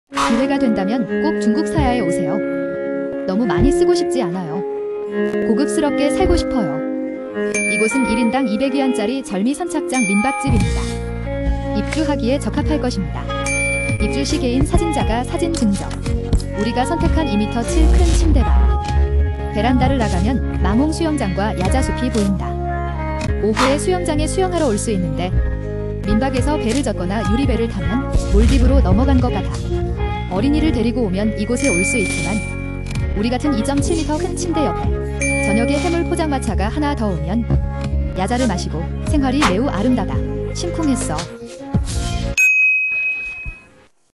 호텔 선택에 도움이 되는 하이난 싼야 여행 및 숙박 가이드
너무 많은 돈을 쓰고 싶지 않고 고급 호텔에 머물고 싶다면 이곳을 선택할 수 있습니다.